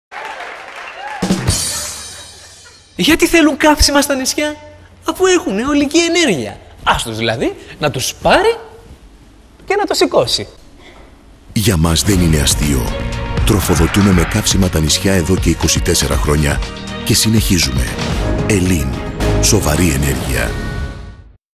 ELIN islands GREEK TV COMMERCIAL
H elin satirizei tin katastasi stin elliniki agora, enimeronei kai prostatevei ton katanaloti. Exipno diafimistiko spot. Ellinika nisia. Energeia. Aioliki. Na tous parei kai na tous sikosei. Greek funny commercial with a stand up comedy twist. The Greek petroleum market and the quality controls of elin. Gasstations, greek islands, wind, sun, cars, fuels.